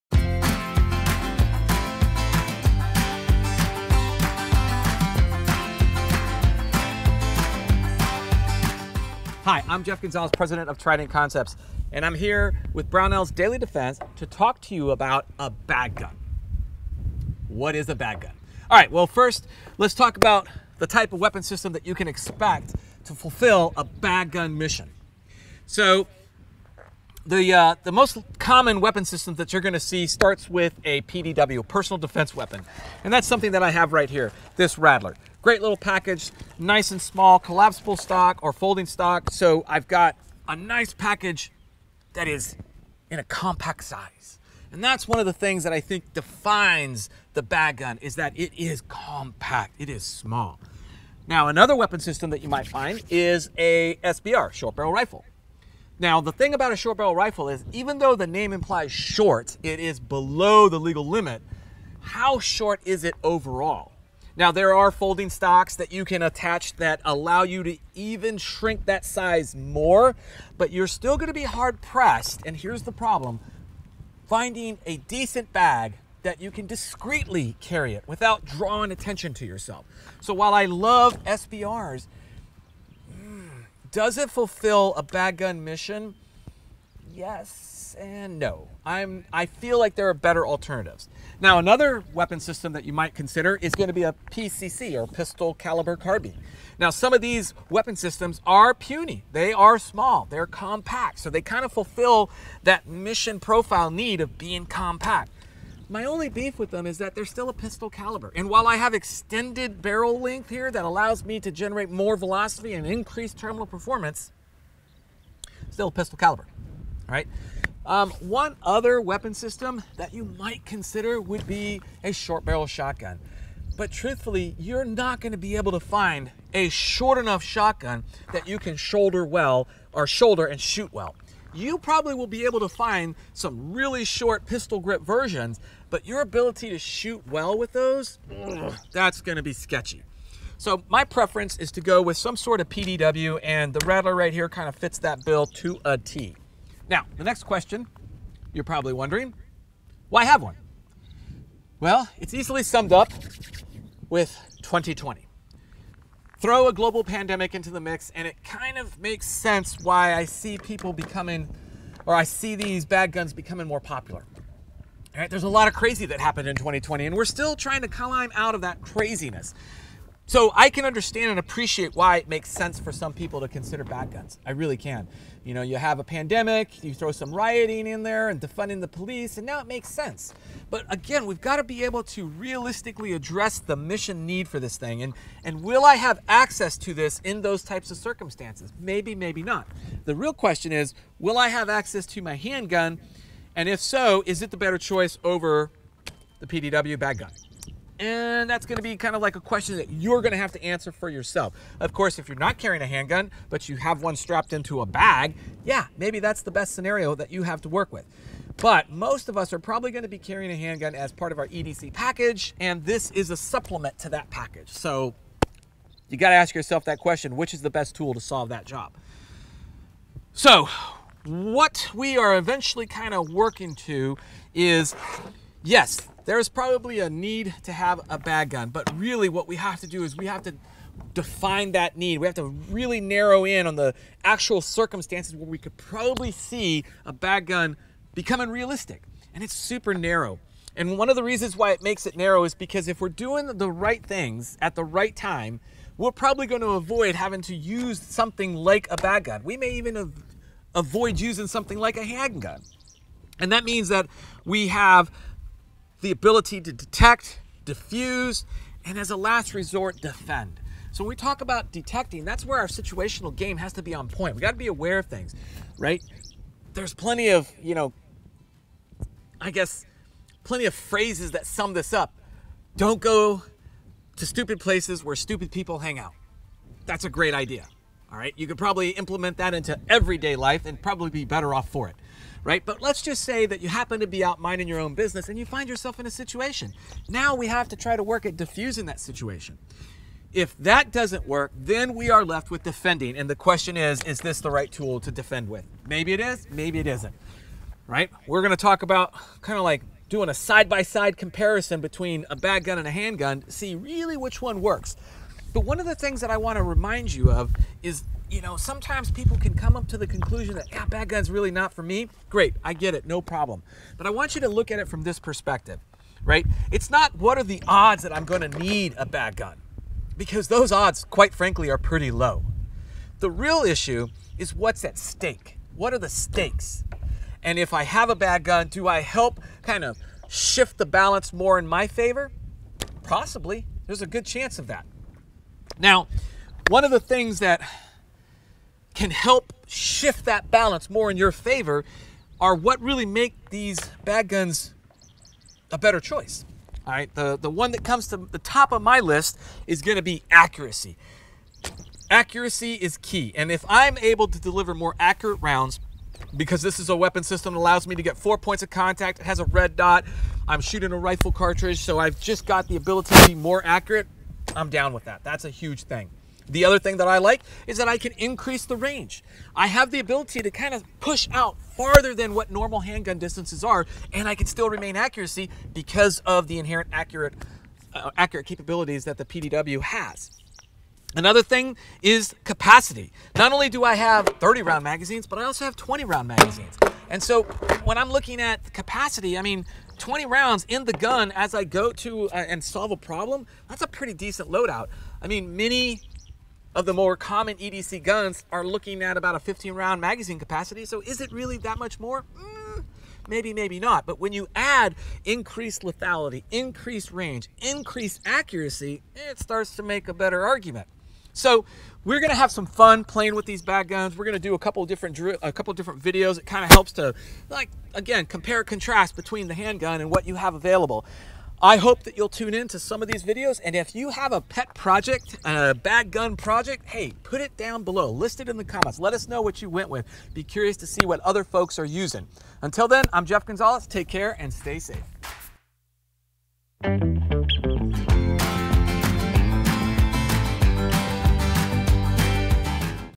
Daily Defense 3-26: What Is a Bag Gun & Should You Get One?
A "bag gun" is what its name suggests: a personal-defense firearm larger than a pistol that is carried discreetly in a backpack or other case that is not recognizable as a gun case. It is simply a concealed carry firearm that provides greater defensive capability than a pistol, which may be needed in situations such as widespread violent civil unrest. Think of it as a supplement to your daily defense handgun, rather a replacement for it. A bag gun needs to be short and compact. It can be a PDW (personal defense weapon), a pistol version of a larger firearm like an AR-15; an SBR (short-barreled rifle), which must be registered with the ATF and may not be legal in your state / municipality; or a small PCC, a pistol caliber carbine. Jeff explains the specific pros and cons of each of these firearms. Some of the benefits of a bag gun over a handgun are better accuracy, longer effective range, and higher ammunition capacity.

Before you go out and get a bag gun, take some time to really define its "mission" - what your actual need for it is. It's a firearm that you would resort to in a very narrow set of circumstances. You'll need to be able to DETECT a genuine threat, then try to DEFUSE the situation, and if that doesn't work DEFEND yourself with the bag gun only as a last resort. The realistic odds of ever needing a bag gun are quite low. But in a bad situation where your life is threatened, what are the stakes? A bag gun can shift the balance more in favor of your survival.